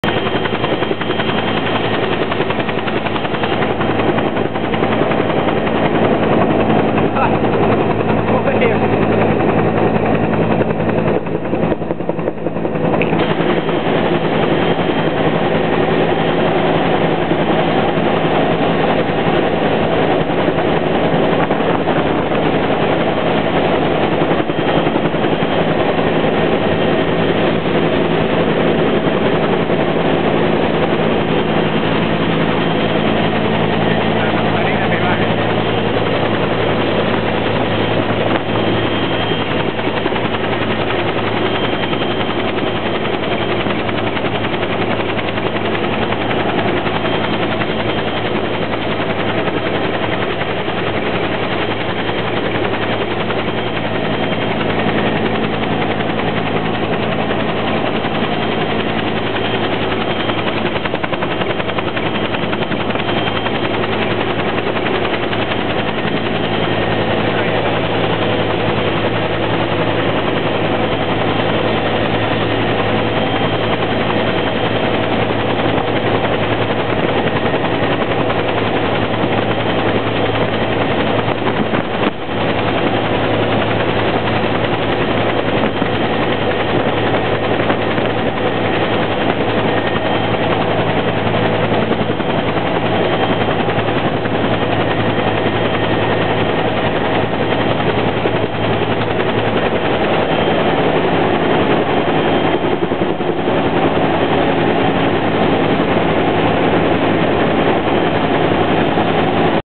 空勤總隊直昇機體驗
其實是民間特搜隊訓練
～我是去插花的～